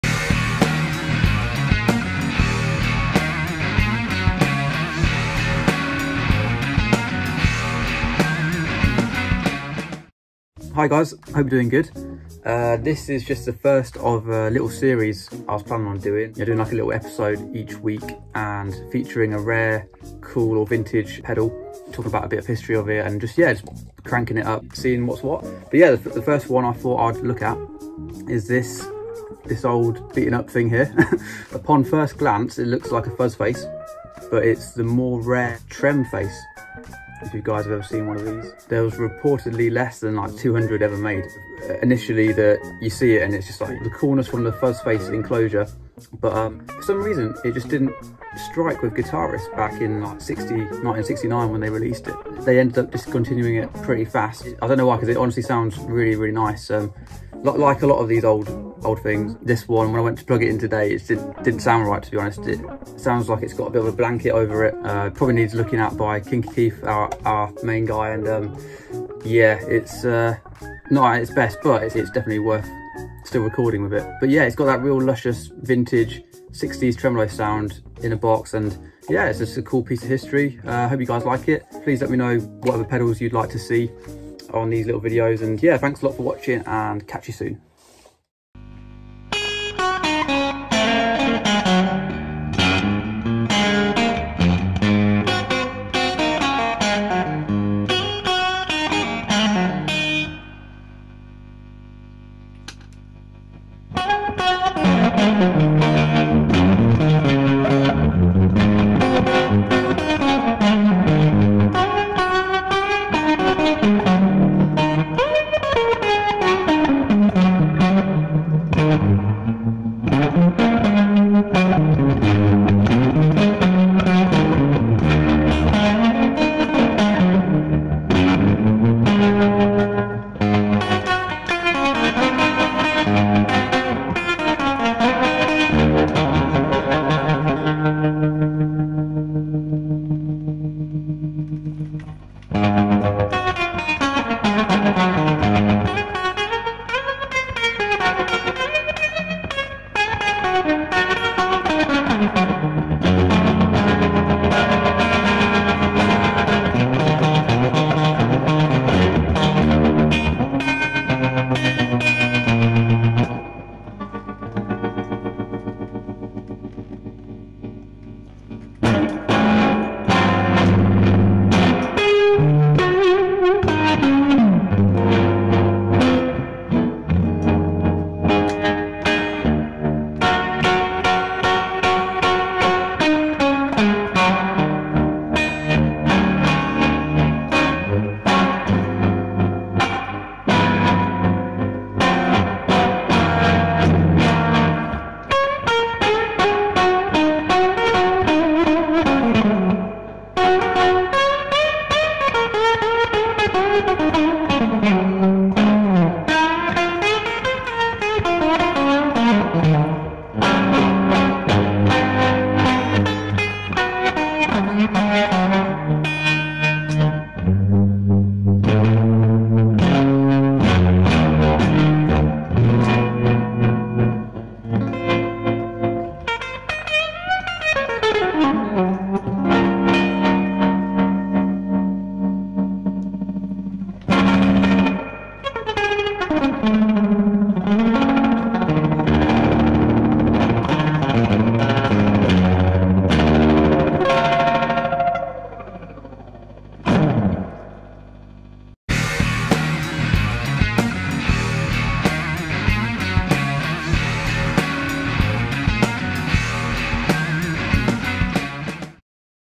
Vintage Fuzz Face... BUT NOT!! | The Pedal Pawn Hub
Welcome to the very first episode of ‘The Pedal Pawn Hub’ - where we take a look at the Vintage 1969 Trem Face...

Thank you all so much for the support! Here is a link to our handmade guitar pedals: